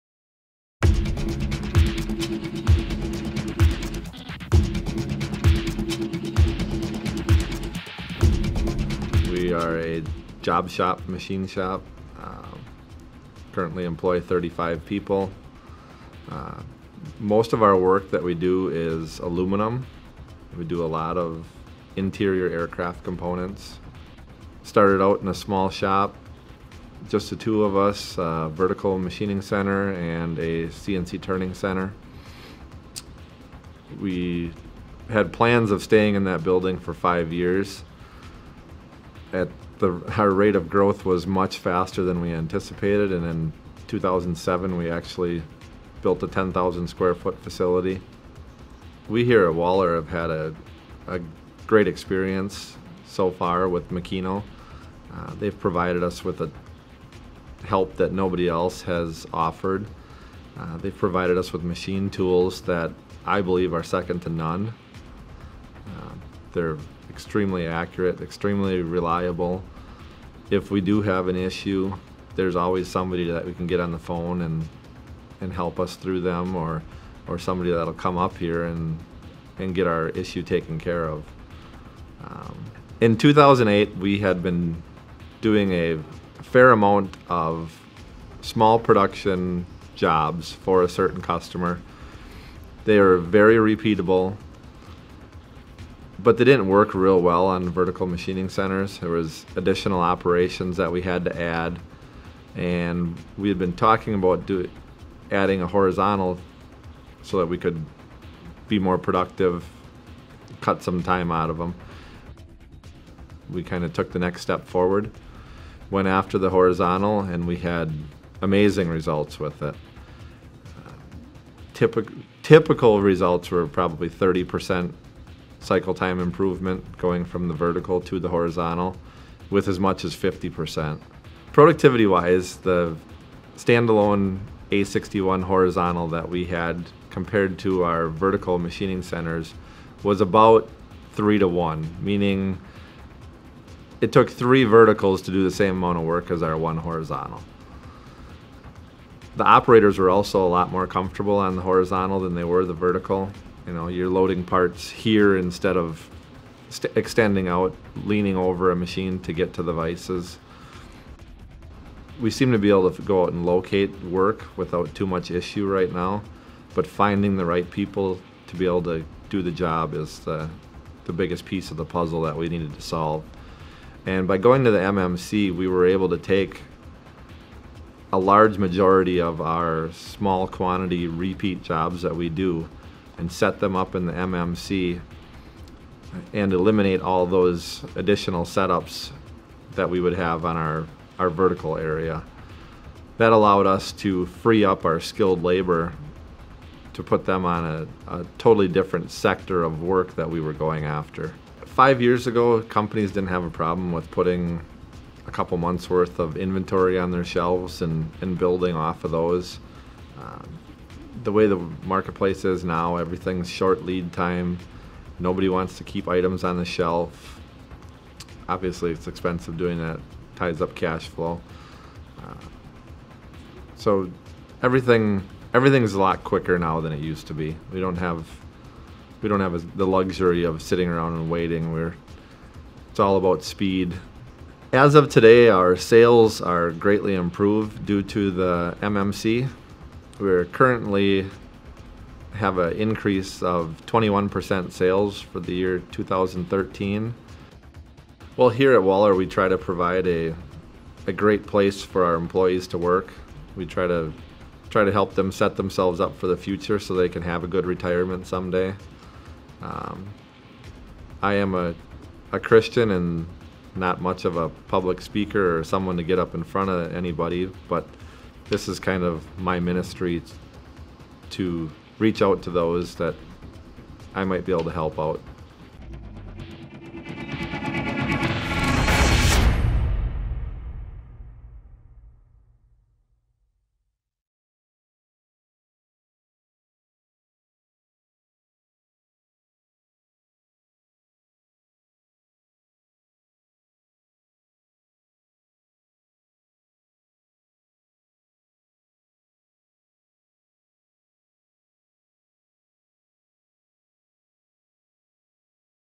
Woller Precision Improves Profitability with Automated Horizontal Machining
To learn more about Woller Precision's use of automated horizontal machining technologies, visit Competitive Production

Aaron Woller, president of Woller Precision Machine, discusses how the company enhanced its productivity and profitability by transitioning from conventional vertical machining centers to an automated horizontal machining cell. Beyond simply improving capacity and workflow, this investment in automation has enabled Woller to maximize the value of skilled labor, redeploying personnel into new business development roles. This practice has led the company to a 21 percent increase in sales, while increasing labor requirements by only 6 percent.